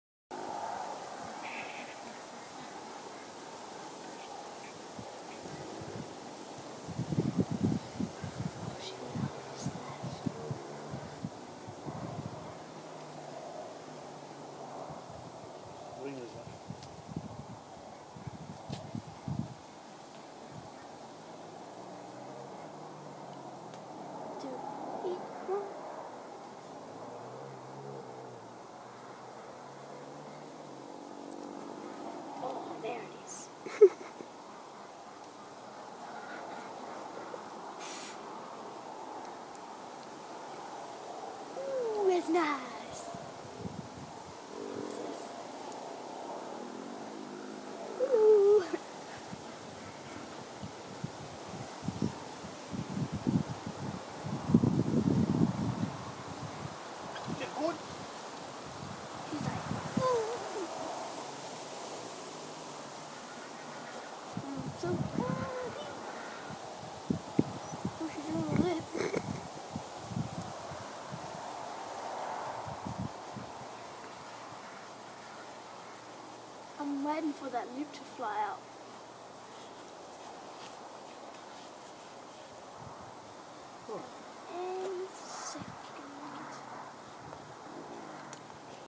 Sticky's back Massage
Giving a pony a back massage before rugging up for a cold night